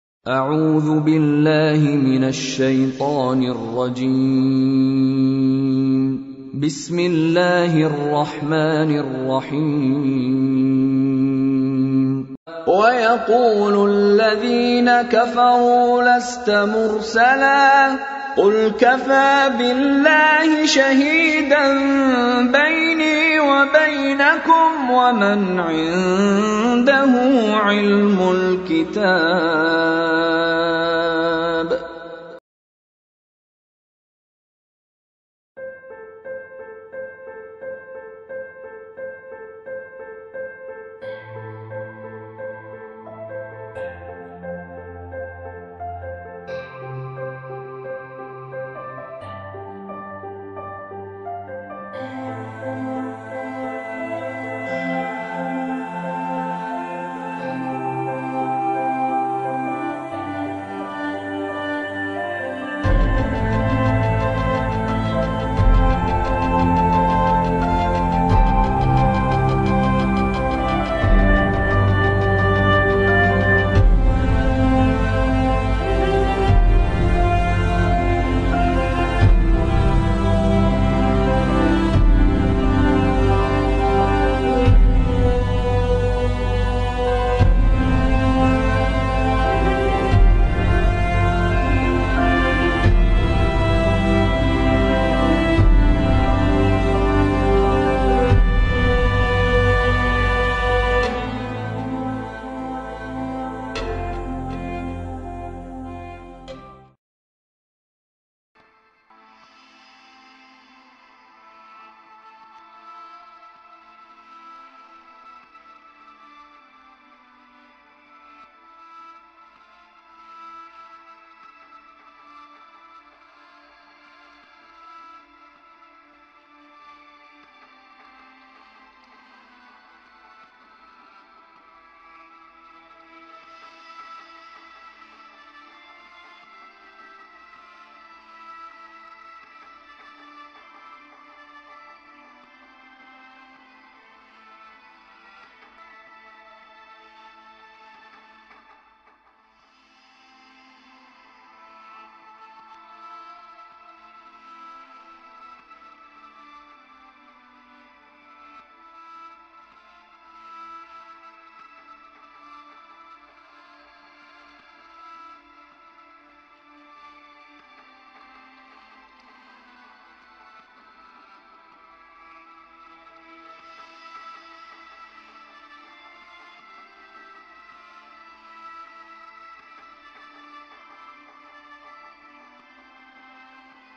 Imam Al Mahdi: أحسن القصص حقيقة أصحاب الرسِّ 1
إمامي الغالي بيّن لي من بيان آيات الله الحقائق المتعلقة بأصحاب الرسِّ وأصحاب الكهفِ -وأصحاب القرية..